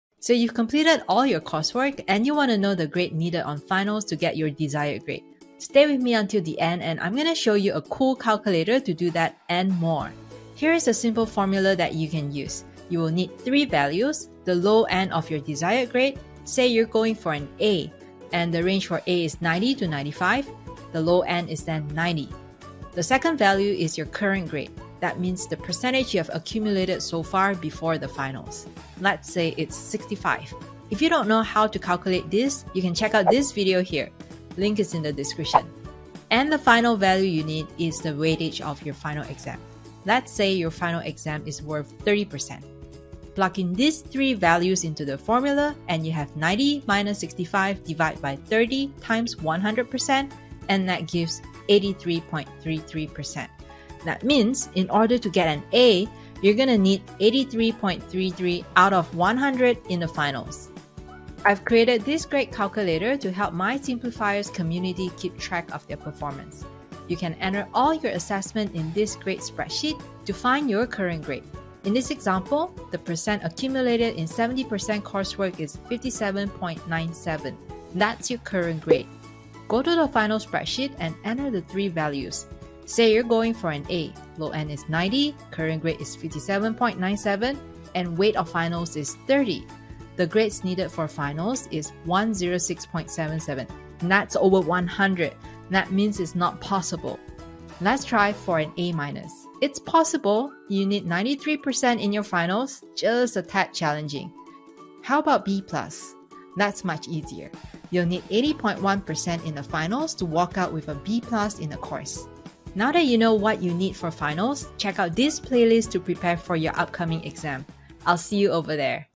Want an A? Want to Pass? Calculate the Grade Needed on Final
Want to know what's the grade needed on the Final to get an A? Pass? Learn how to calculate the grade needed on the final exam using this simple formula. Join SIMPLIFIERS for the free calculator!

QUESTION❓
Have a question about exam preparation, study tips, and strategies, or anything else? Post in the comments section of this video!

ABOUT:
In this video, you're going to learn the simple formula to calculate the grade needed on finals to get your desired grade. At the end of the video, I'll share a simple grades calculator that I've built for my SIMPLIFIERS community.